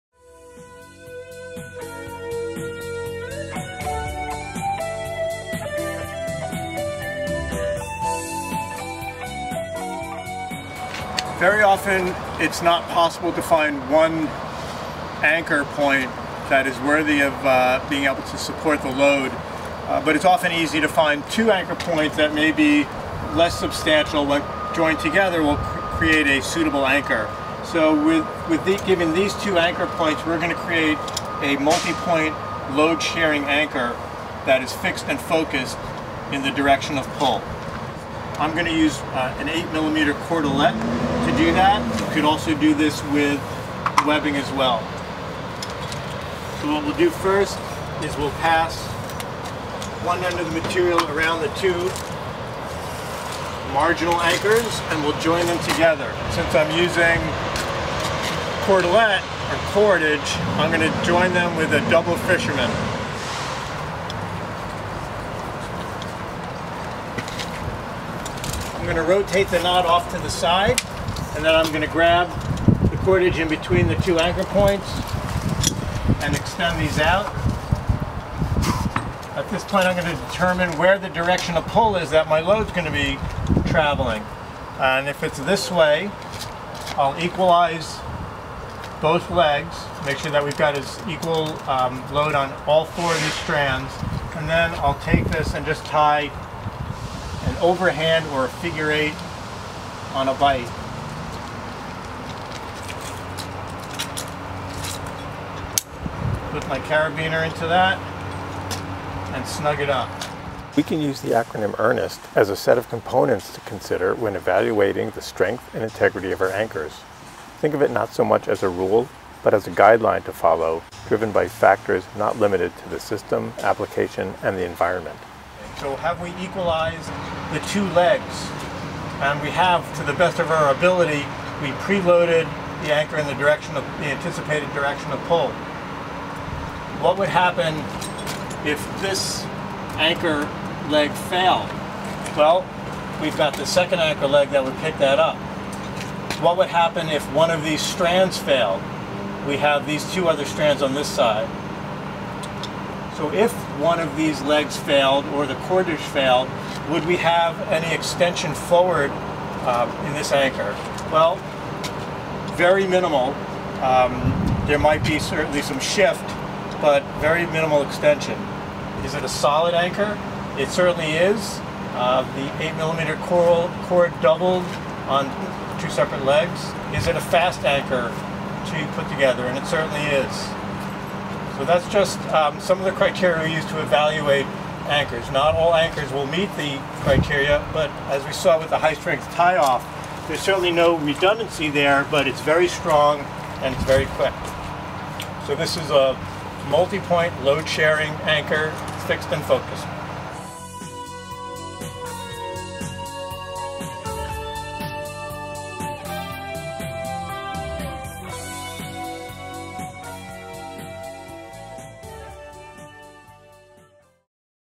Load Sharing Anchor: Rope Rescue, VTTF1 USAR
Multi-point load sharing fixed and focused anchor.